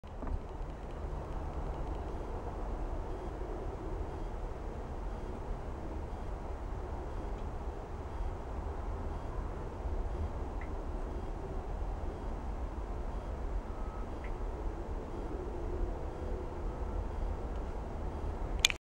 Peachy Peach Little Tree Air Freshener in Denver Junkyard
A Peachy Peach Car-Freshner Little Tree air freshener in Denver self-service wrecking yard, fall of 2014.